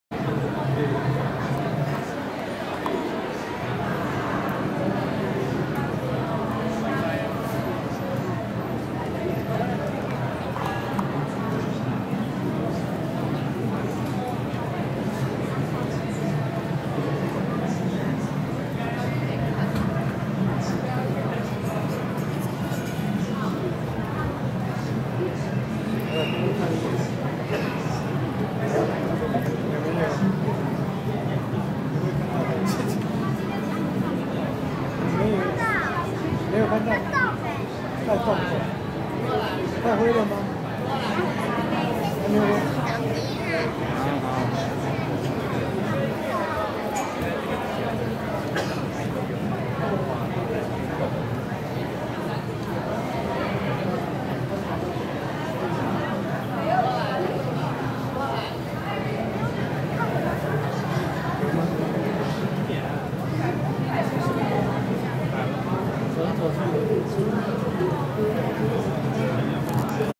望眼鏡下的風景
宜蘭蘭陽博物館用望遠鏡看蘭陽平原生態。